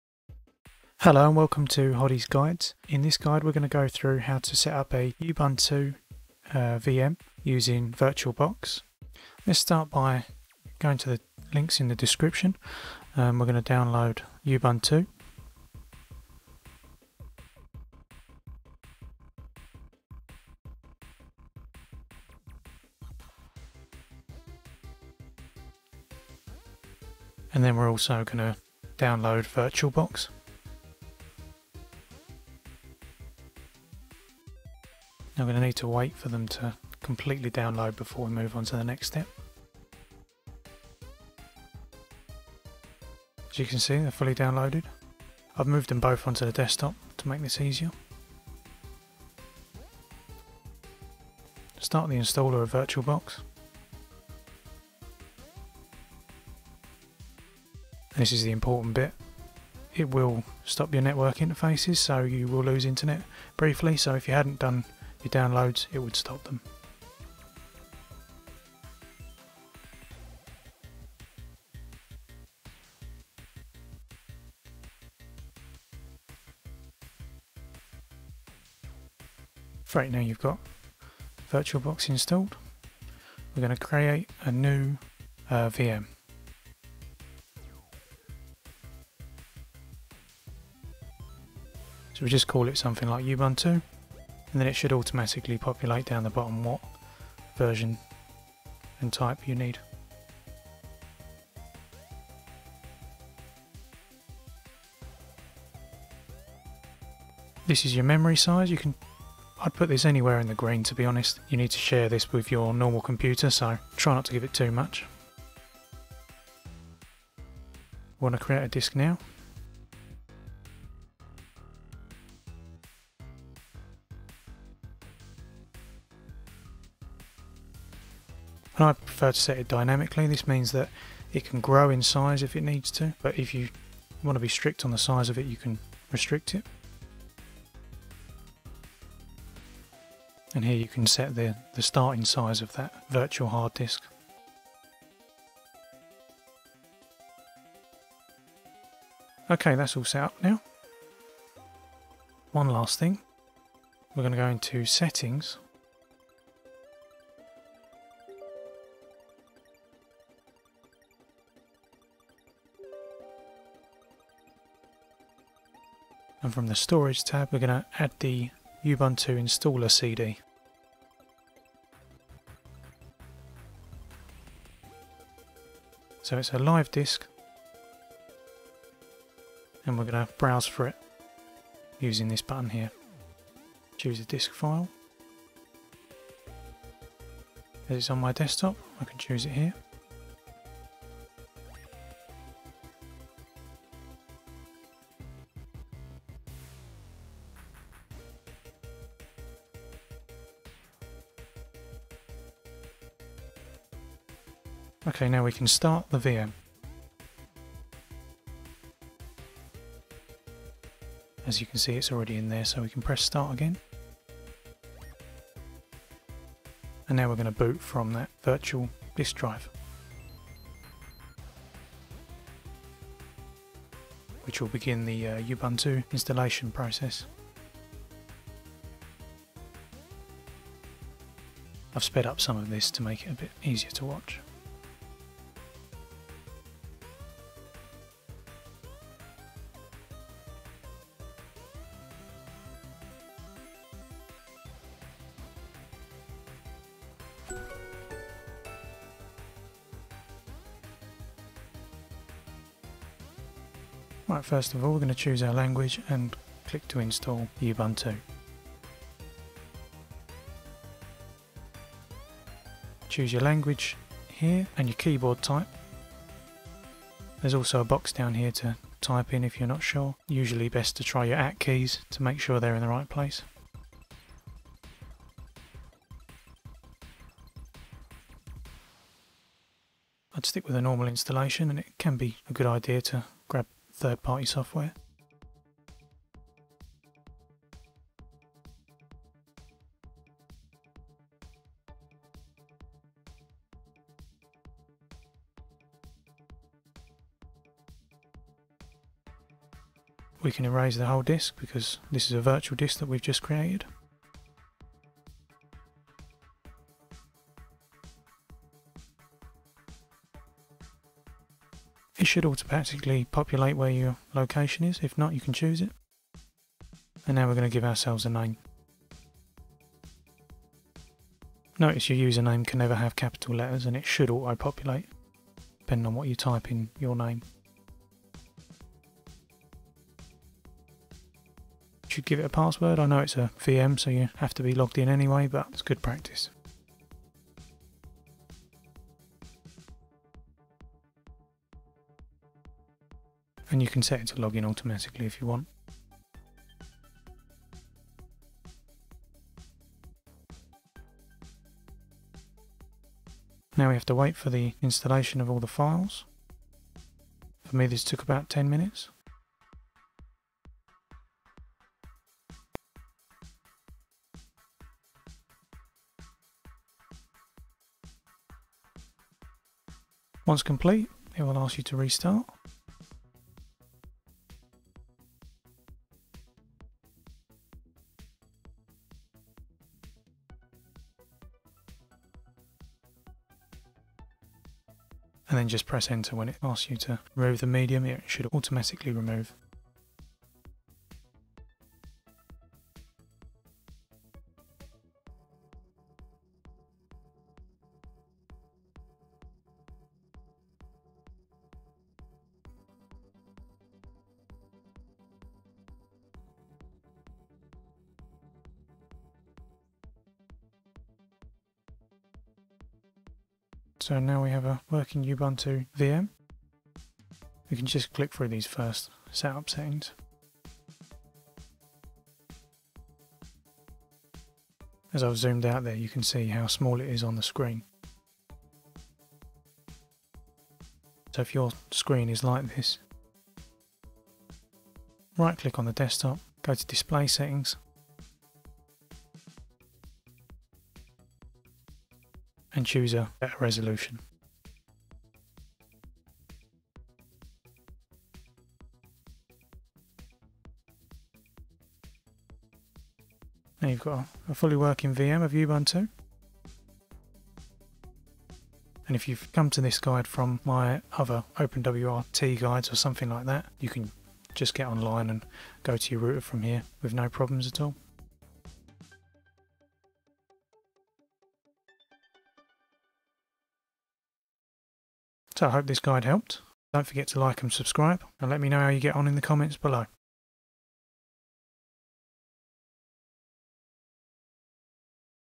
Setting up Ubuntu VM for Windows
Setting up Ubuntu 20.04 LTS Virtual Machine for Windows.

Chapters:
00:10 Downloads
00:50 Virtual Box install
01:19 Creating the VM
02:46 Adding the install .iso
03:25 Start and install Ubuntu
07:01 Changing screen resolution
7:34 OpenWrt